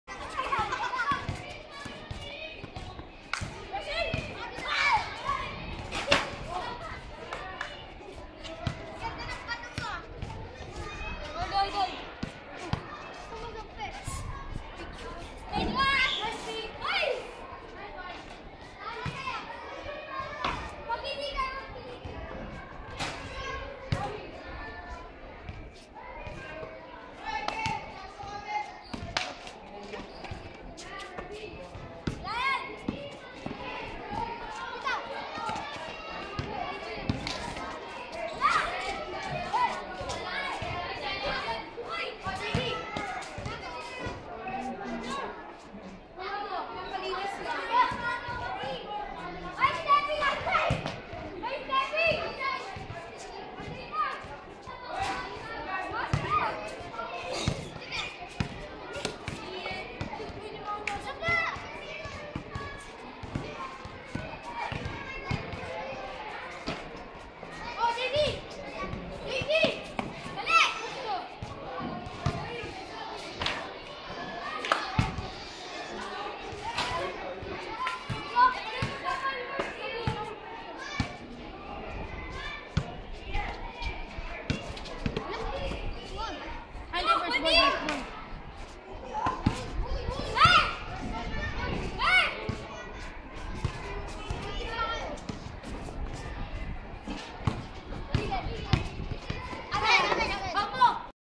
2013 VSJA Family Day - Video 5
Some basketball action during Family Day at VSJA,